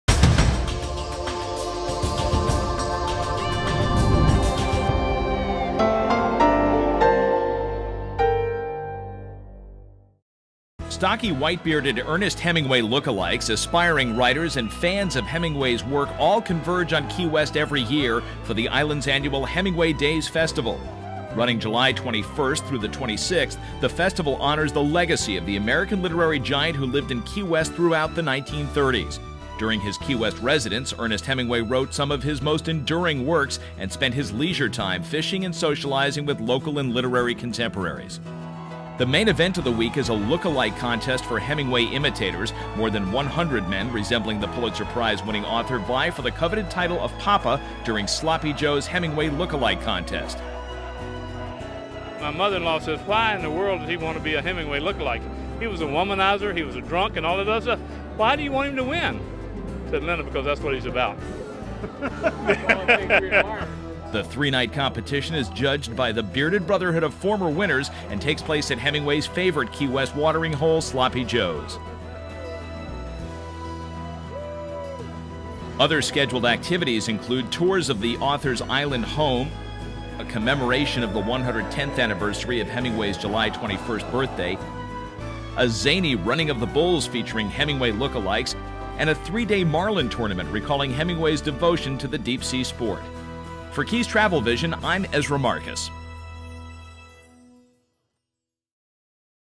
Hemingway look-Alike Contest in Key West
Key West's annual Hemingway Days festival honors acclaimed author Ernest Hemingway, who lived on the island during the 1930s. The main event of the week is a look-alike contest for Hemingway imitators.